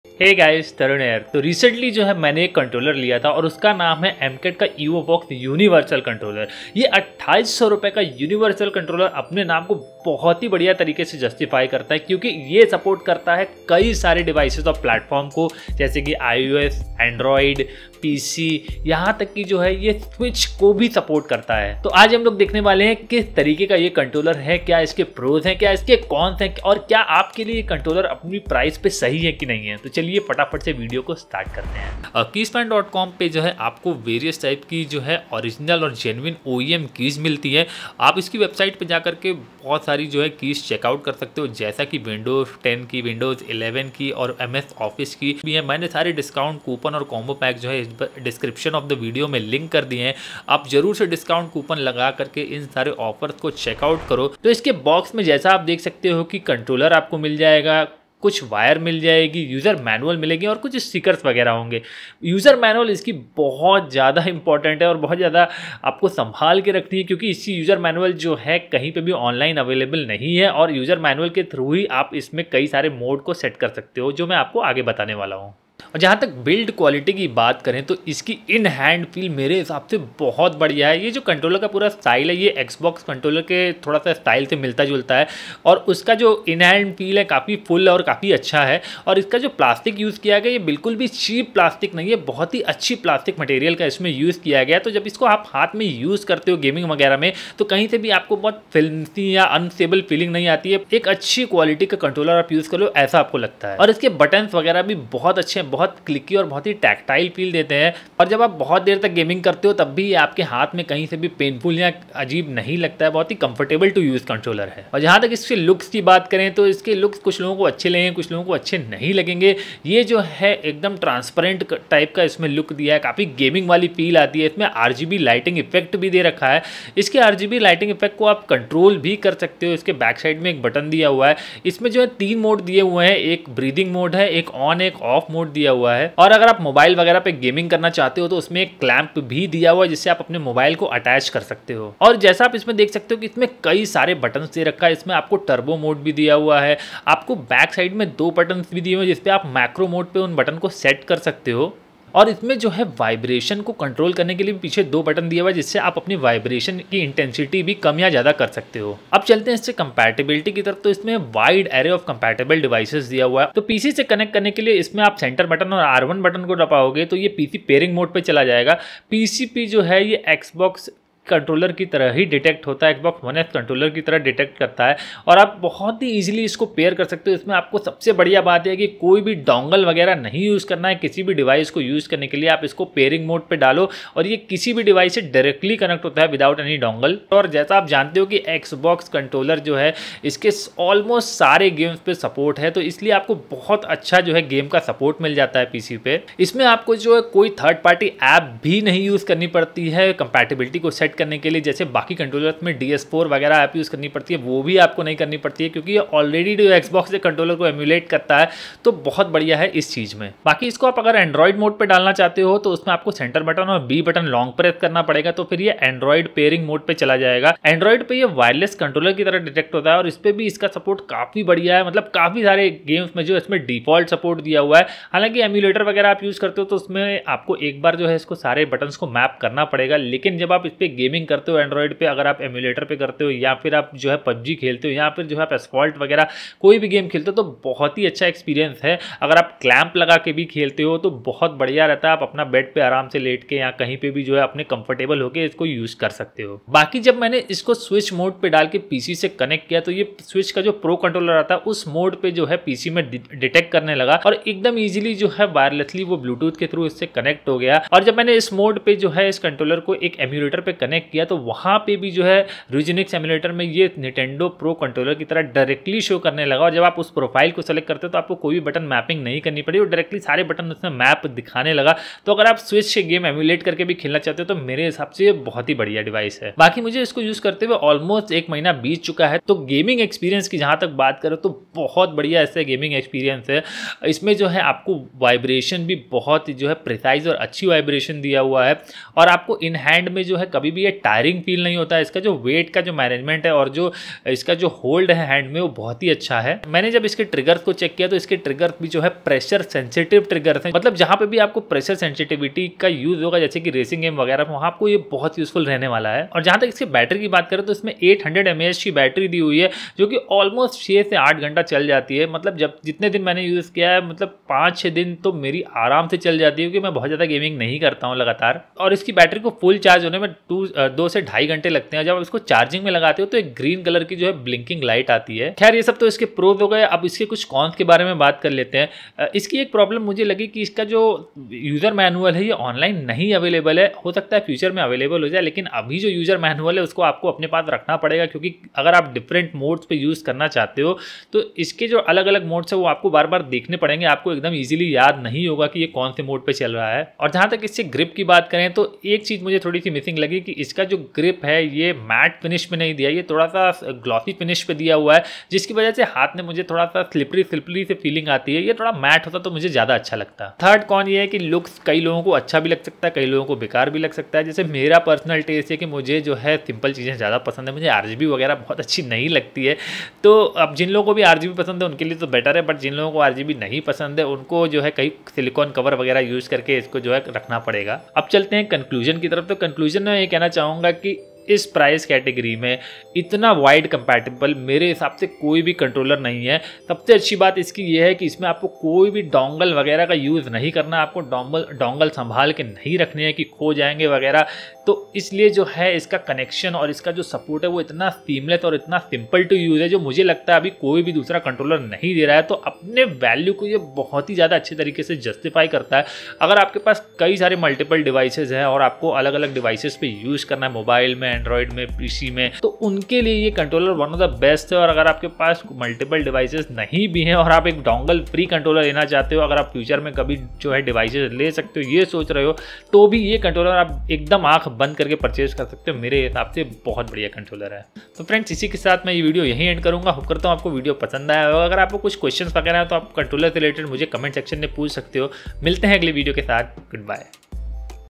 Amkette Evofox One Universal Gamepad Review and Unboxing | Hindi
Amkette EvoFox One Universal Bluetooth Gamepad for iOS, iPadOS, Android, PS4 and PC (X-Input) with The Dojo App, Detachable Mobile Clamp, Low Latency Bluetooth 5.1

Chapters :

0:00 Intro
0:31 Sponsors
0:55 Unboxing
1:15 Build quality and design
2:39 Compatibility (with pc)
3:32 With android
4:06 Switch mode
4:37 Gaming experience
5:06 Battery life
5:27 Cons
6:18 Verdict

Get Windows and Office easily and cheaply

Use of any part of Video without proper permissions is not allowed .